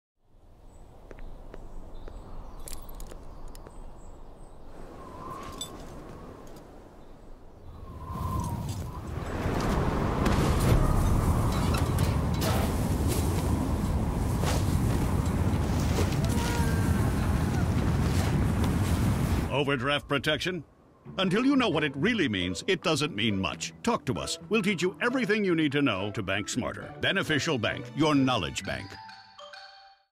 Beneficial Bank "Overdraft" Campaign Television Spot
Brownstein Group helped Beneficial Bank launch its "Overdraft" campaign with this 30 second television spot